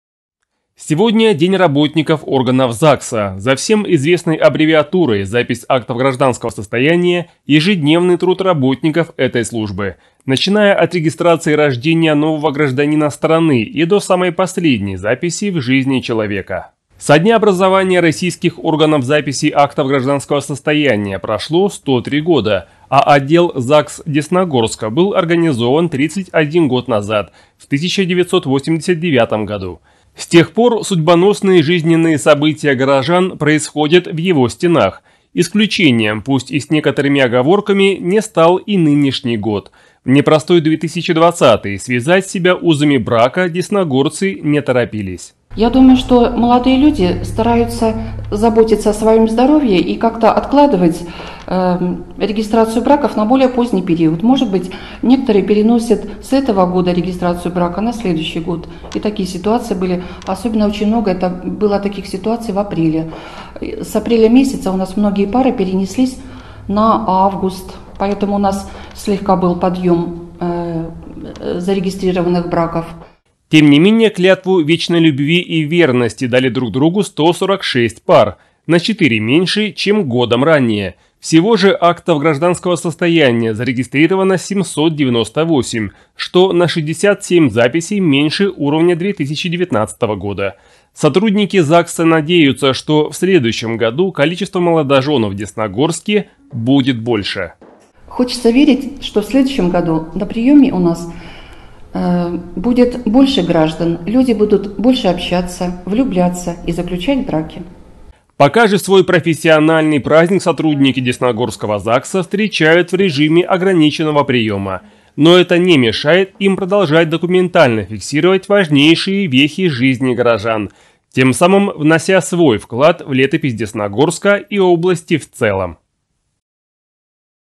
Десна-ТВ: День работников органов ЗАГС
18 декабря - День работников органов ЗАГС. За всем известной аббревиатурой - запись актов гражданского состояния – ежедневный труд работников этой службы. Начиная от регистрации рождения нового гражданина страны и до самой последней записи в жизни человека.